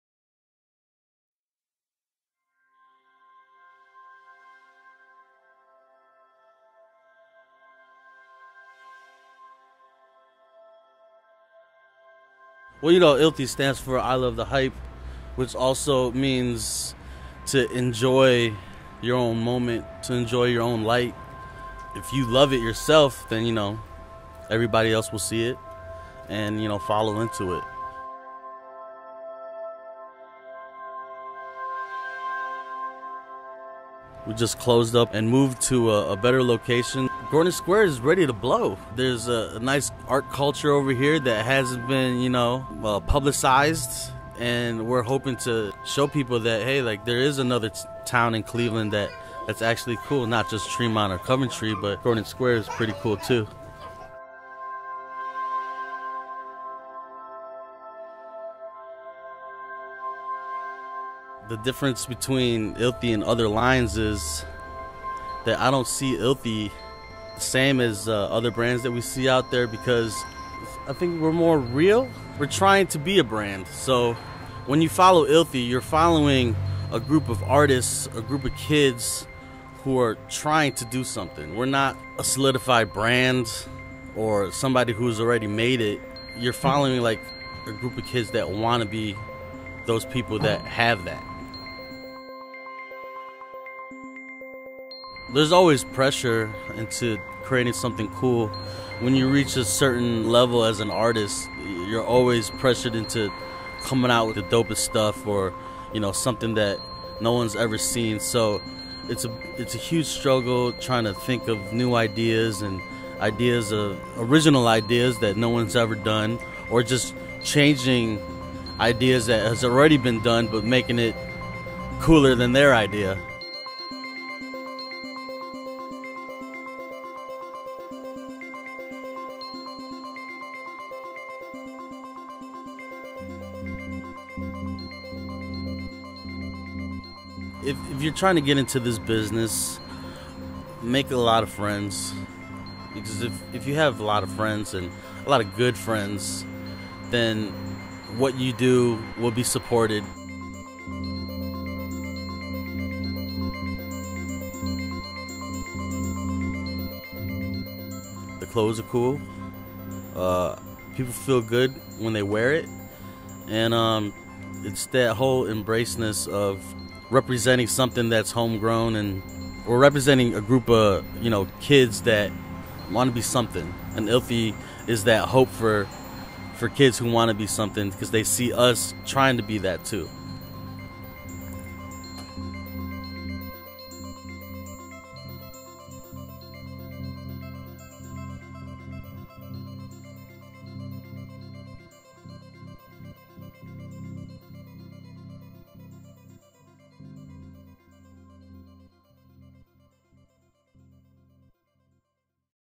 iLTHY | New Location
iLTHY has moved to a new location. Check out a first look into our new building and the process that we go through during our move. Narrated by founder Glen Infante.
Grand Opening
iLTHY Workshop 6602 Detroit Ave, Cleveland, Ohio 44102
December 15th, 2012
4pm til 7pm
Special edition live silk screened shirt will be given to the first 50 customers with an apparel purchase.
Video by Purple Films
Featuring: Glen Infante, Gregory Schade, Jordan Morales, Amy Narodovich and Kathia Something or other.
Cameos by Brett Vance, Christian of C&M Signs, 2Pac Shakur and Biggy Smalls
Thank you Gordon Square for allowing our team of misfits into your lives.
Shout out to the best cinematographers in the midwest, Purple Films, Jimmy Gall, Daniel Gillespie, and Brett Vance